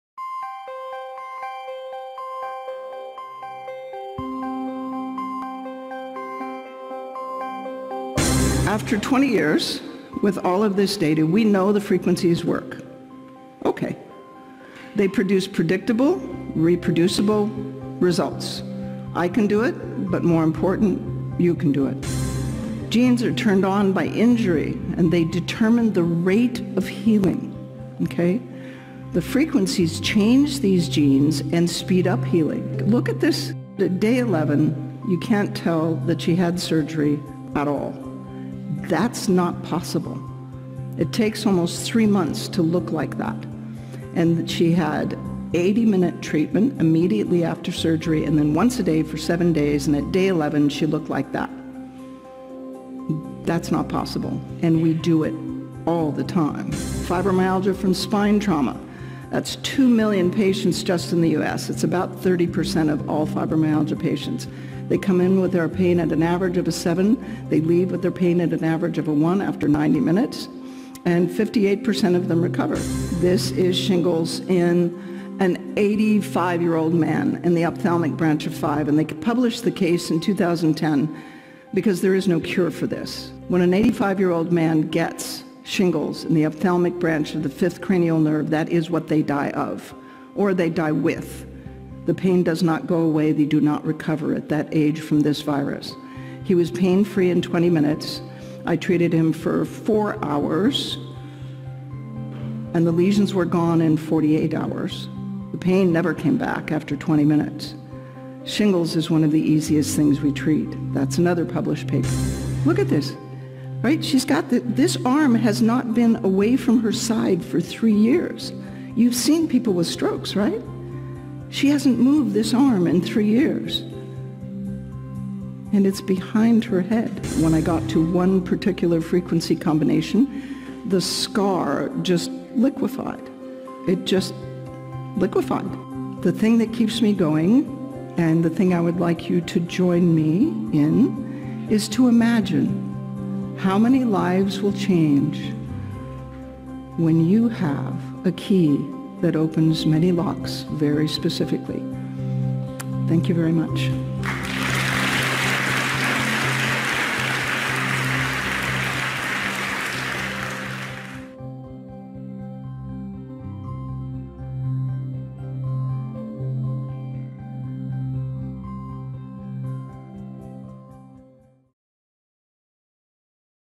Dr Carolyn McMakin über FSM in der Medizin
Frequenzspezifischer Mikrostrom (FSM) bei akuten und chronischen Schmerzzuständen und bei Verletzungen führt zu einem erweiterten Verständnis von Verletzungen, Entzündungen und Schmerzen. Spezifische Frequenzen können die Heilung von verletzten Sportlern beschleunigen, in dem sie:
- ​​​​​​die Wundheilung nach Verletzungen beschleunigen​​​​​​​​​​​​​ sollen
​​​​​​​- Schmerzen reduzieren sollen
​​​​​​​- Narbengewebe vermindern und den Bewegungsradius verbessern sollen
- ​​​​​​​Muskelverhärtungen auflösen sollen
​​​​​​​- Mineralische Ablagerungen nach alten Verletzungen auflösen sollen.